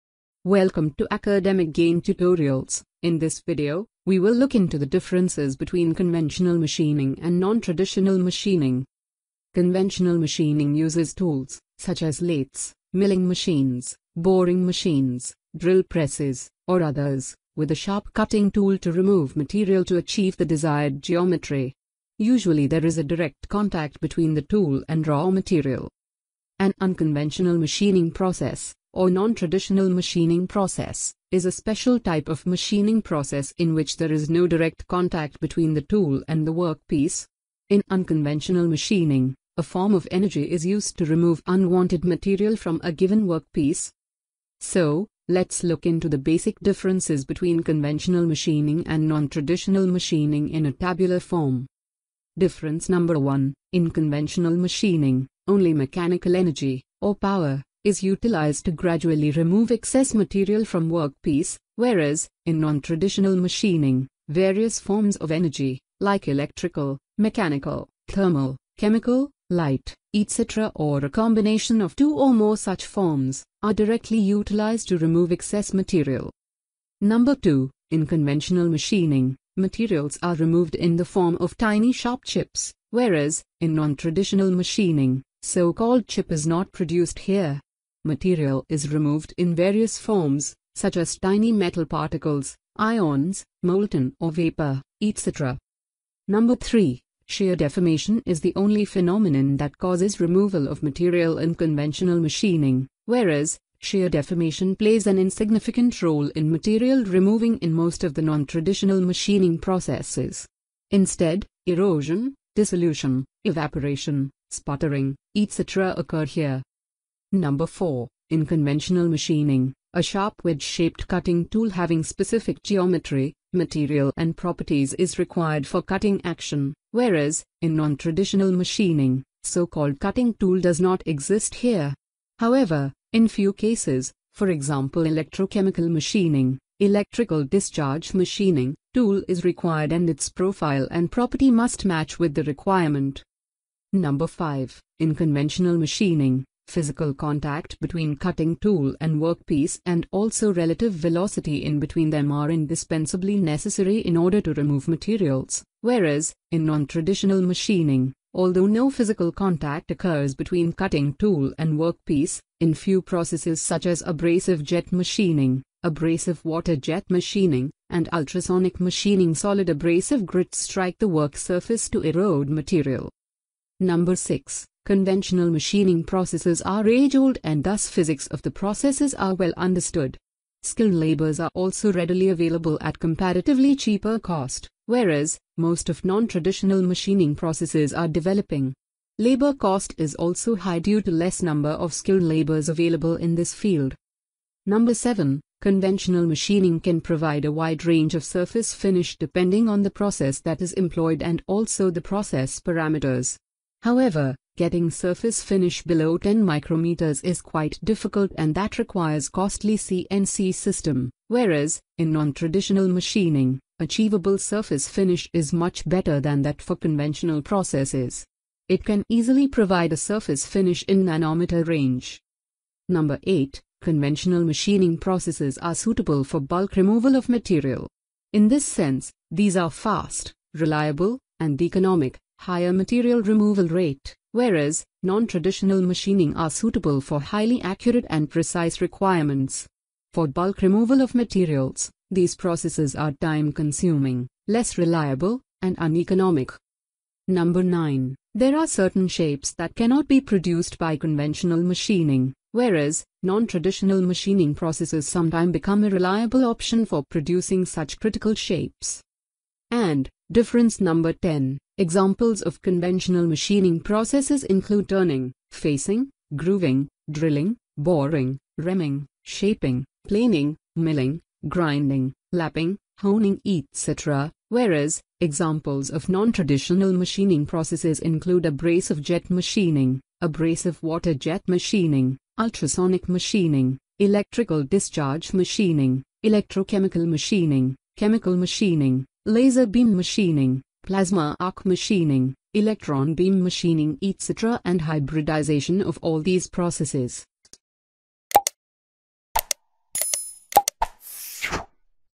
Differences between Conventional Machining and Non-Traditional Machining (NTM).
This video covers a detailed discussion on the major differences between Conventional Machining and Non-Traditional Machining (NTM).